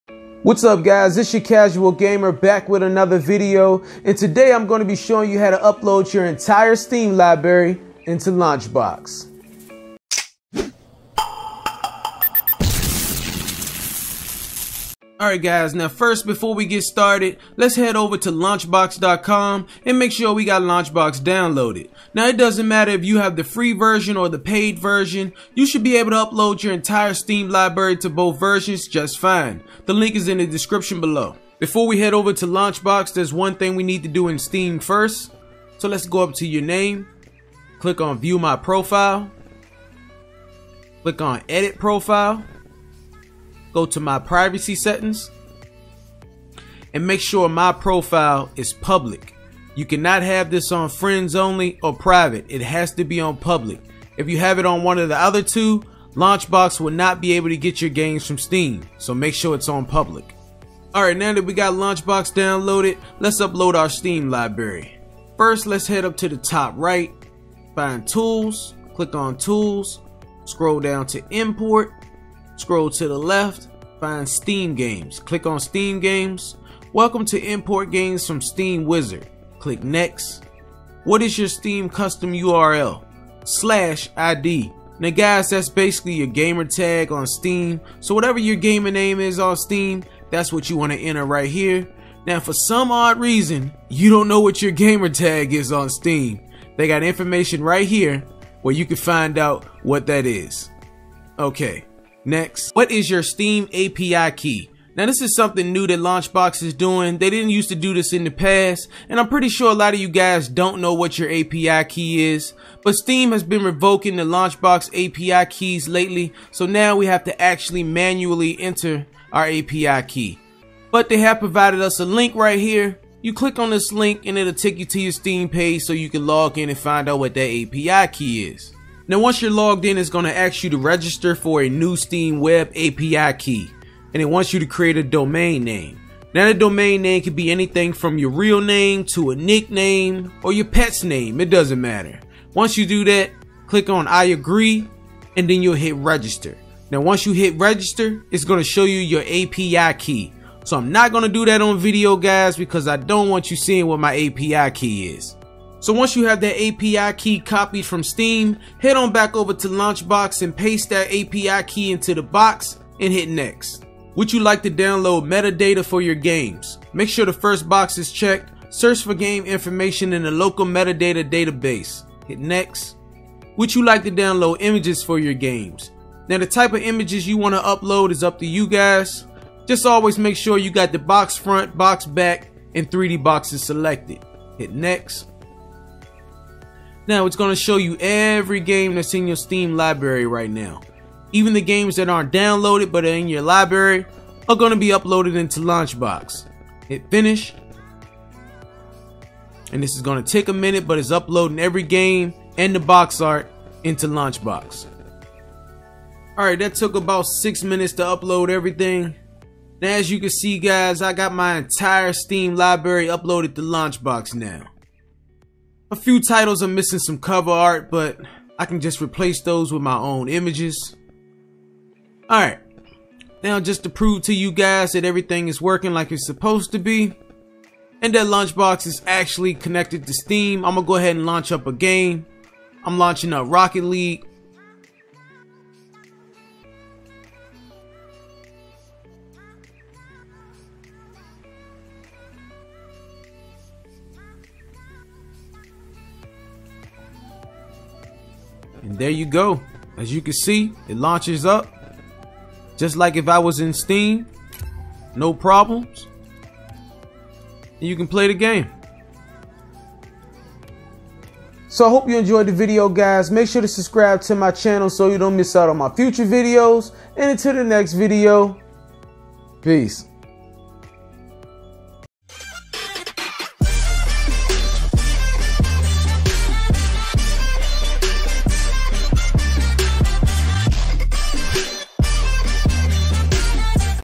How to add Steam games to LaunchBox
This is how you install your entire steam liberty to LaunchBox in 2020